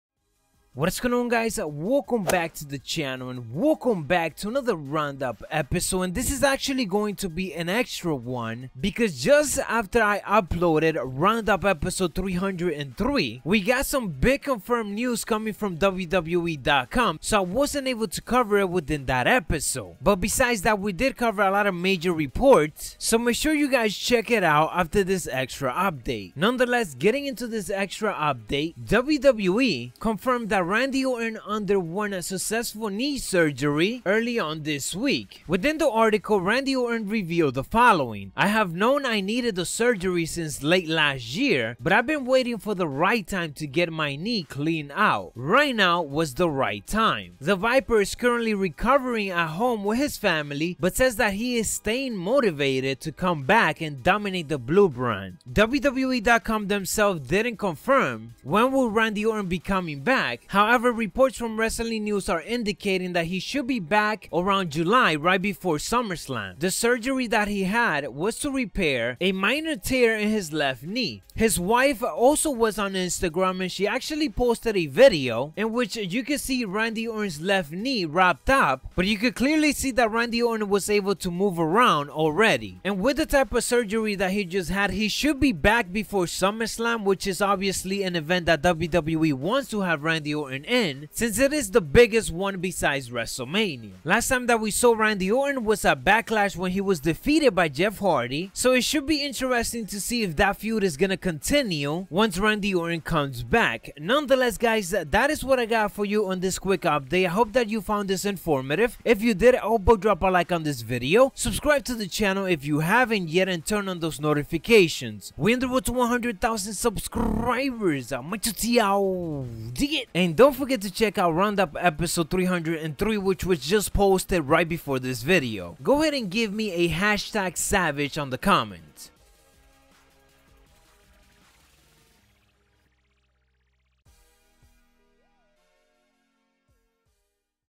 TOP SMACKDOWN SUPERSTAR OUT OF ACTION After Successful Knee Surgery, & For How Long? -The Round Up
TOP SMACKDOWN SUPERSTAR OUT OF ACTION After Successful Knee Surgery -The Round Up...Do Not FORGET!

- Macho T